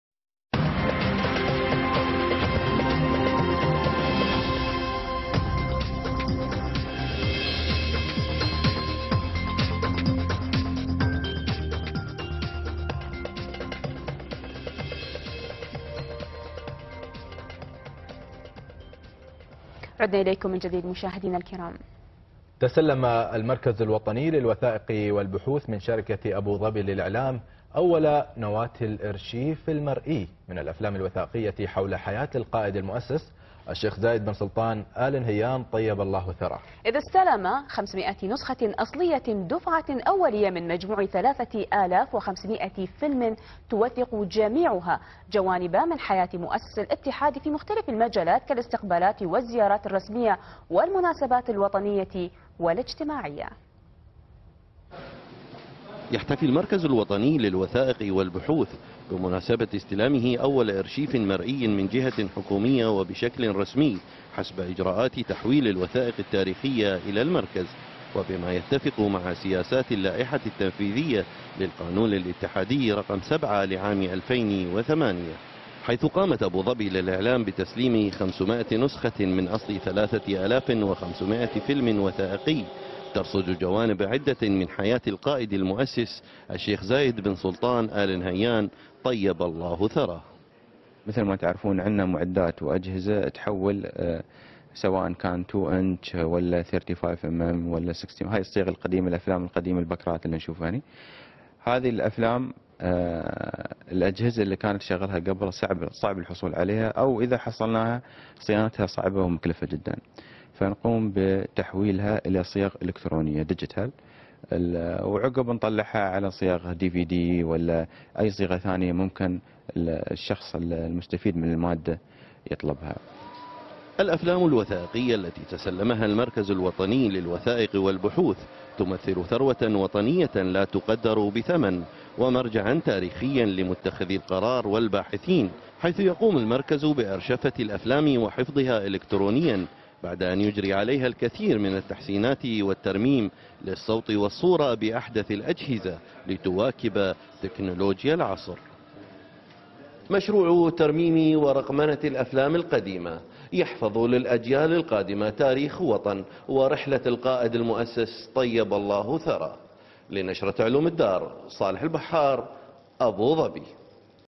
الأرشيف الوطني يتسلم مئات الأفلام الوثائقية التاريخية من أبوظبي للإعلام
الأرشيف الوطني يتسلم الدفعة الأولى من 3500فيلم وثائقي عن الشيخ زايد من أبوظبي للإعلام

Source from ADTV